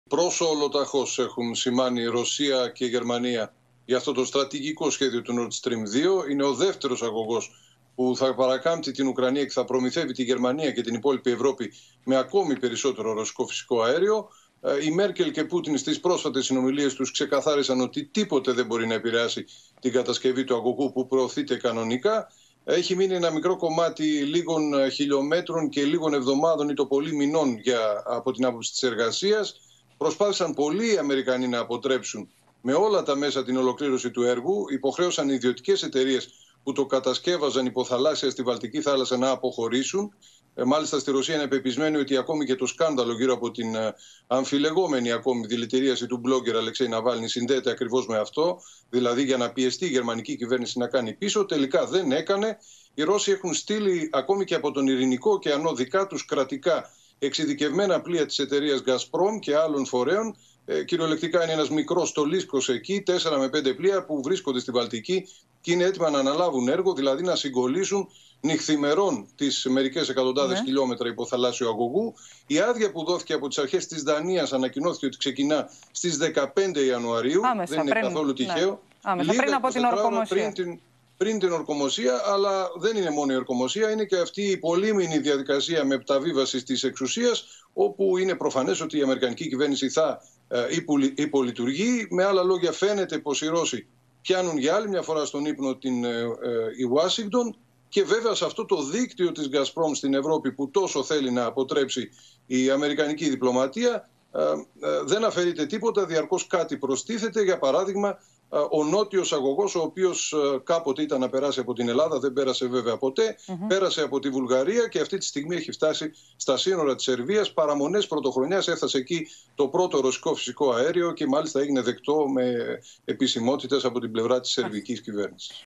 «Φουλάρει» η Μόσχα για τον αγωγό Nord Stream-2 που δεν θέλουν οι ΗΠΑ | OPEN TV
«Φουλάρει» η Μόσχα για τον αγωγό Nord Stream-2 που δεν θέλουν οι ΗΠΑ | Κεντρικό Δελτίο Ειδήσεων 8/1/2021 | OPEN TV

Πρόσω ολοταχώς από Ρωσία, αλλά και Γερμανία, για το στρατηγικό σχέδιο του Nord Stream-2, τον δεύτερο αγωγό, που θα παρακάμπτει την Ουκρανία και θα προμηθεύει τη Γερμανία και την υπόλοιπη Ευρώπη με ρωσικό φυσικό αέριο.